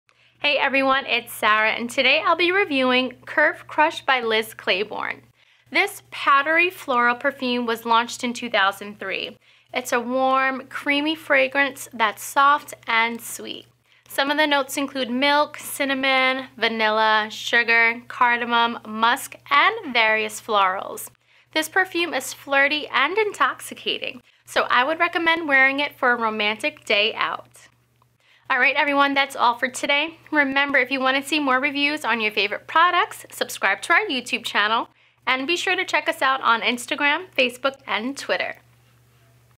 Curve Crush Perfume by Liz Claiborne Review
Sarah reviews Curve Crush by Liz Claiborne. This flirty fragrance is soft and sweet.